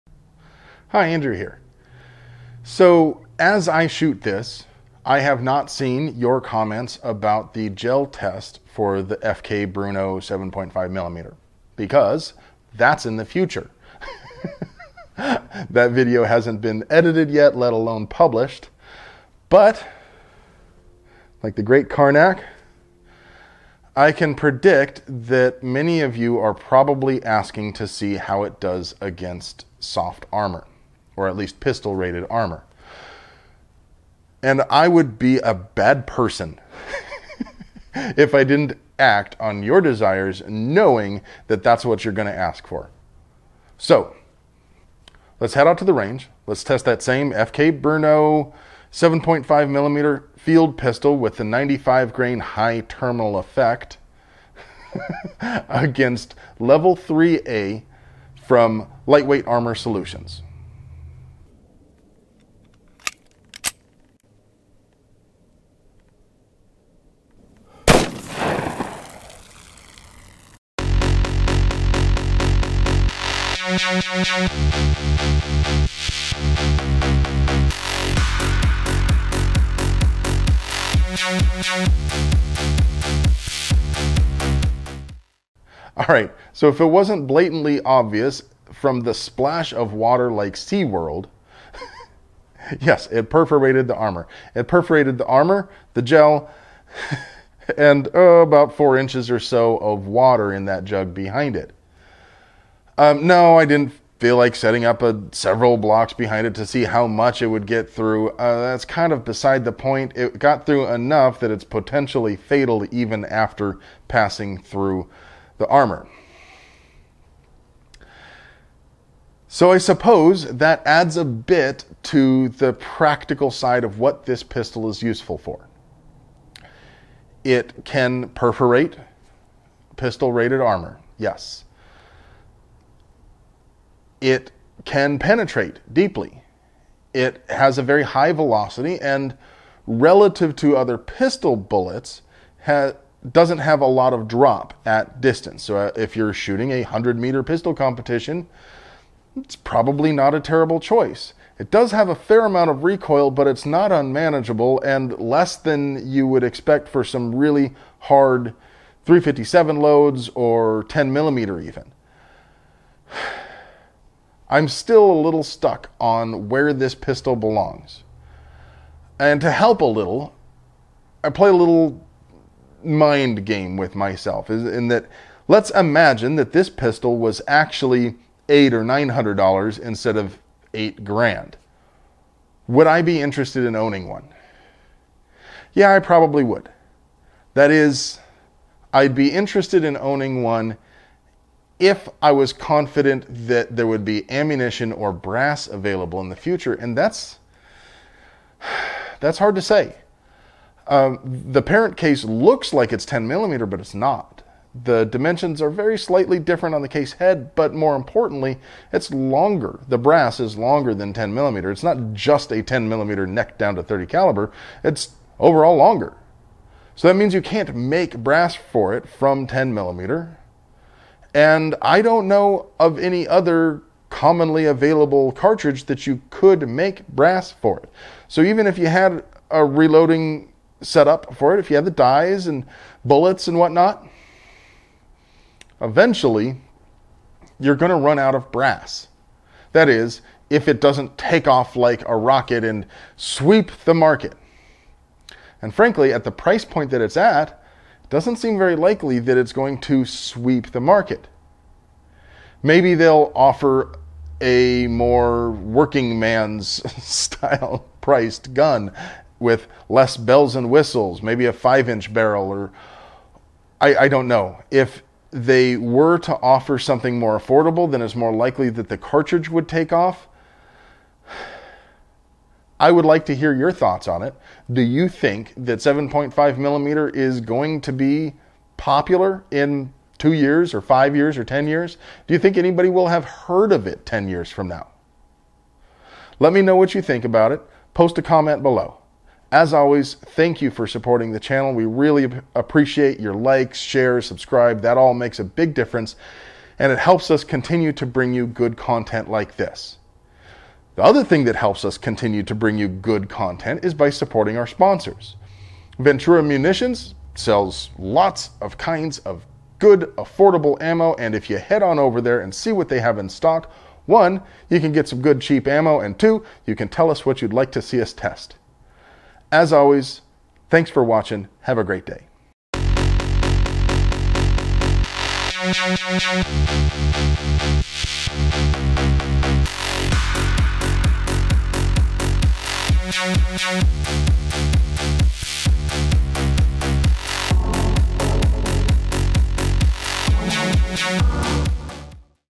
Can the "World's Fastest" Production Pistol Defeat Armor? FK Brno 7.5mm vs. IIIA Armor
The FK Brno 7.5mm Field Pistol is a high end boutique pistol based on the CZ75 and chambered in a rather intriguing proprietary cartridge. At first glance, it seems almost like 9x25mm Dillon, except necked down to .30 caliber, rather than .35 caliber. But it is longer than 9x25mm Dillon and 10mm Auto. Instead, it seems that it might be derived from their common parent case, the .30 Remington.

It produces seriously impressive velocities at over 2,000 fps:

2,009
2,030
2,026
2,017
2,041
Average: 2,024.60
StdDev: 12.26
Min: 2,009
Max: 2,041
Spread: 32

So with these velocities, it ought to perform well against pistol rated armor, but I have been surprised before. Velocity is the primary factor in defeating pistol rated armor made of aramids, polyethylene, or steel. But bullet shape and integrity does play a role. This bullet has a somewhat rounded ogive and fragmented readily in ballistic gel. Will that have any impact on armor performance?

As it turns out, no. The 7.5mm honey badgered right through a level IIIA hard aramid plate like it wasn't there. This is not indictment on the armor, of course. That same panel from that same manufacturer was able to stop .44 magnum and some pretty warm 10mm. But at 2,000fps, you'll likely need legit, rifle rated armor to stop this cartridge.

So where does that put the pistol and the cartridge? The cartridge is seriously intriguing. I'm a sucker for innovation. A legit 2,000 fps in a "real" pistol is pretty neat. The ammo isn't even ridiculously priced, all things considered. Sure, a buck per round is not cheap, but it's also a brand new cartridge loaded with solid copper bullets. To keep it in perspective, compare it to .357 Sig defense loads with the Barnes TAC-XP.

But that's where things start to fall apart, if you'll pardon the pun. FK chose to introduce this pistol and this cartridge with a load they dub "high terminal effect". When we tested this ammo in ballistic gel it fragmented early and left a rather small, caliber sized track that continued on almost a yard.

But after the first 9" or so, we are left with a .30 caliber sized wound track. That sounds worse than it is. Bear in mind that the closest comparison, solid copper hollow points in .300 AAC, also leave a big TSC, followed by a relatively small track. The difference is that a 110-120 grain .300 AAC solid copper HP tends to have an even larger and longer TSC and the lower velocity track is larger than caliber because those bullets expand and retain their petals, cutting a wider track.

I would have liked to see the debut of this cartridge with an expanding projectile with good weight retention and less than 20" of penetration. That isn't an insurmountable task, either. There are plenty of bonded lead core and solid copper hollow point projectiles that can hold together and expand well in this velocity range. Bottom line on the cartridge is that it has incredible potential for defensive use that wasn't fully realized in this projectile design. Despite that shortcoming, this load is still rather impressive.

The pistol itself is expensive at $8,000 MSRP and I'm not convinced it earns that price tag. I don't think I've ever seen an $8,000 pistol that wasn't a Title II device, but I've handled plenty of $3,000 custom 1911s and the like. The fit and finish on the FK Brno is close. It is indeed quite finely made in some respects.

All edges are attractively radiused. The fit between parts is tight, and there isn't any rattle when you shake it. It feels like one, homogeneous unit when the slide is in battery. The FK Brno feels good in the hand. It fits well, despite the long cartridge overall length and my stubby carny fingers. The extended slide stop and safety are easier to manipulate than the standard versions on other CZ clones and the CZ75 itself. The butterfly style sight is unusual but you can quickly adapt to it. With practice, it could give a great balance between speed and accuracy. You can focus on the front sight and roughly align with the rear for speed, or allow greater attention to rear sight alignment as time permits. If you are not fond of the sight, you can replace it with a more traditional notched rear or even a mount for a mini red dot.

Guns in this video:
FK Brno 7.5mm Field Pistol

FIGHTS HARD, WEARS EASY